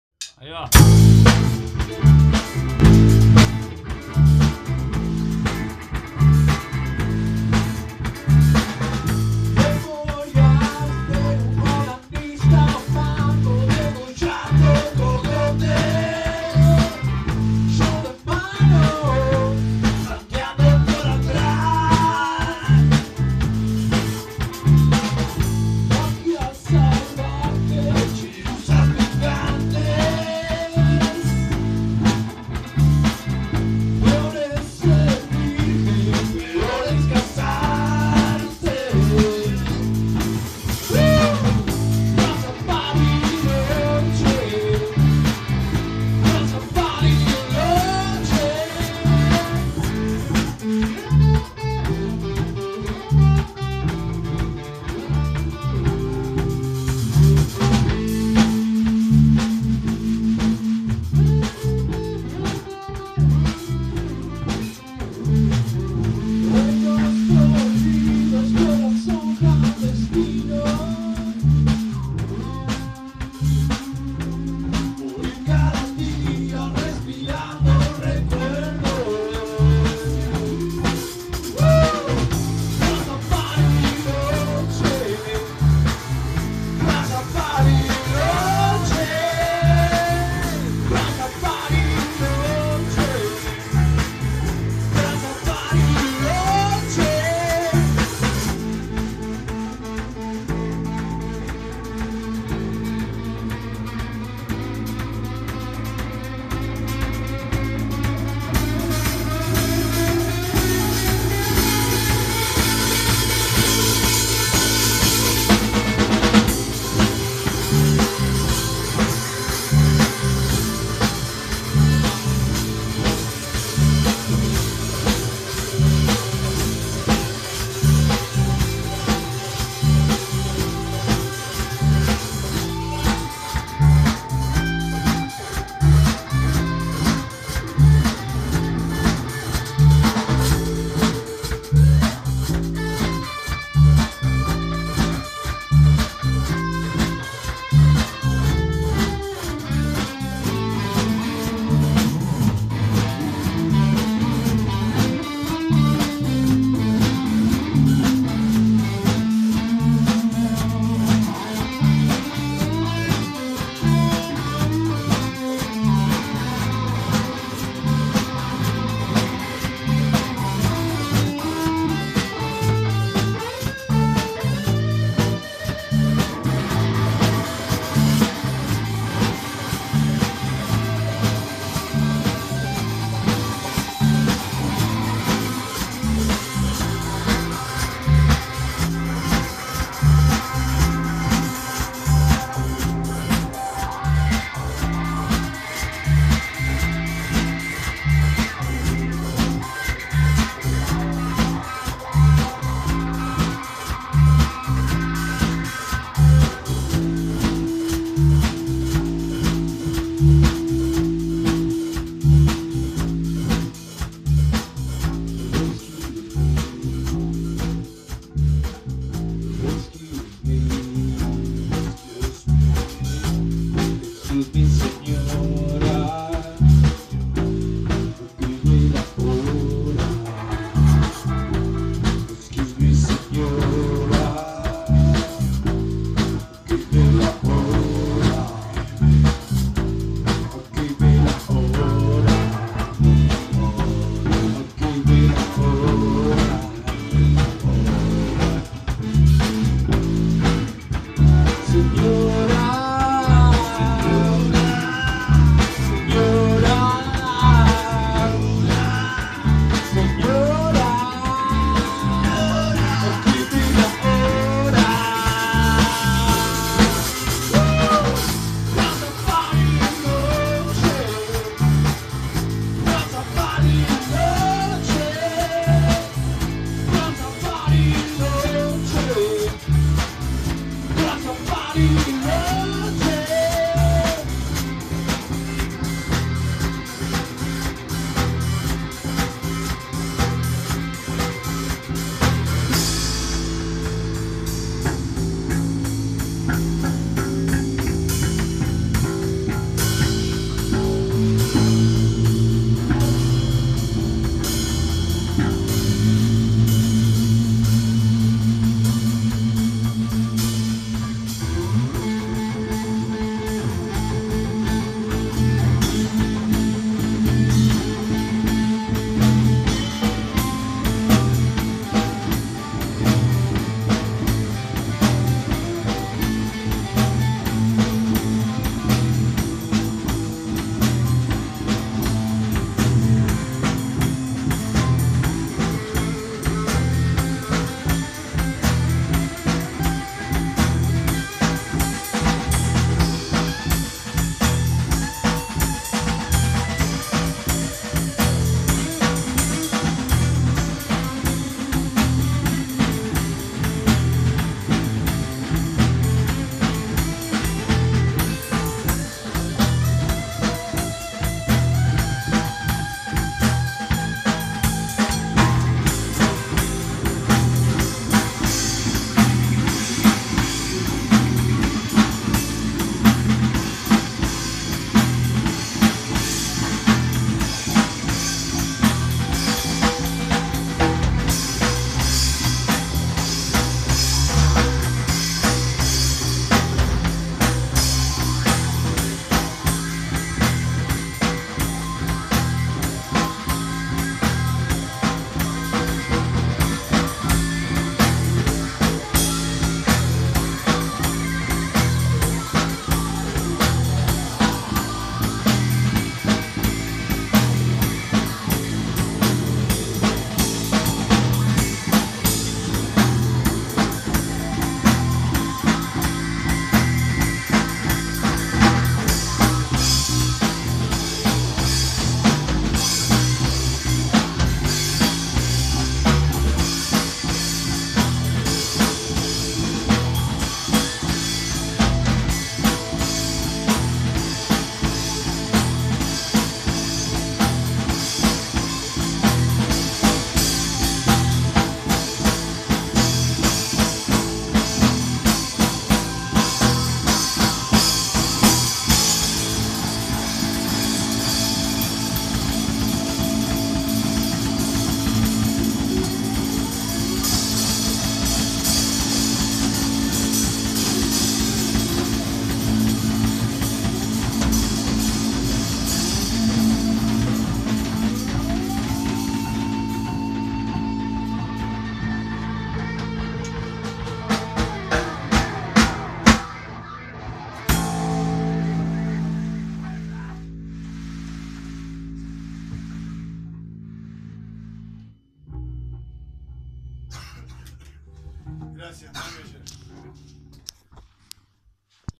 EL GORDO ELVIS ensayo de "TRANSA BARILOCHE" 02/10/2018
Ensayo en "LA SALITA"
BARILOCHE, ARGENTINA
MARTIN GOL: VOZ Y BAJO
MAXI RICCHETTI: GUITARRA Y COROS
JOCHY GALASSI: BATERIA Y COROS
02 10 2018